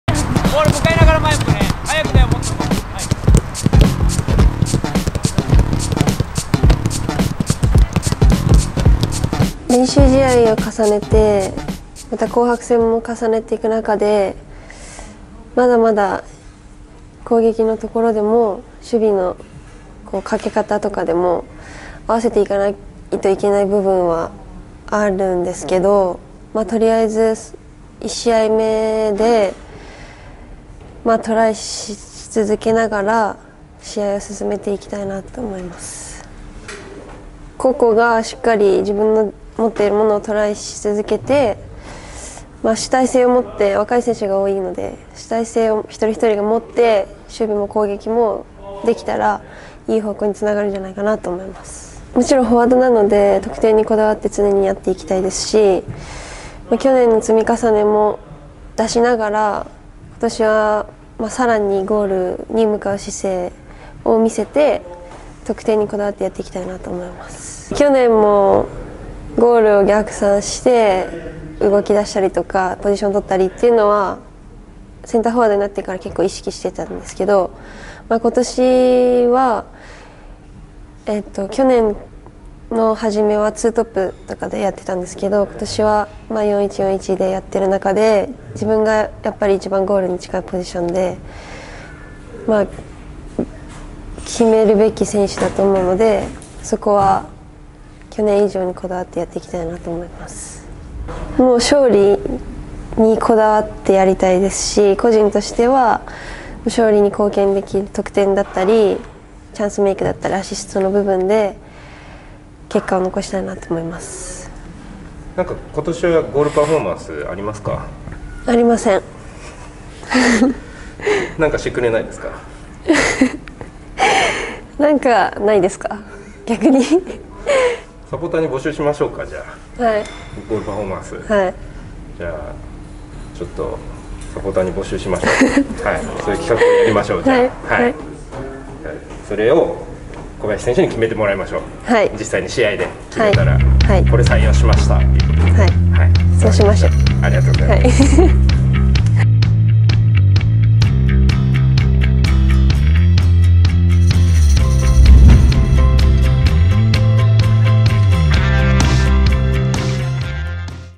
【BELEZA TV】今季初陣直前！小林里歌子選手のインタビュー
今季初陣直前！
小林里歌子 選手 のインタビューをお届けします。

リカコにやってほしいゴールセレブレーションを募集中！
まもなく14時キックオフ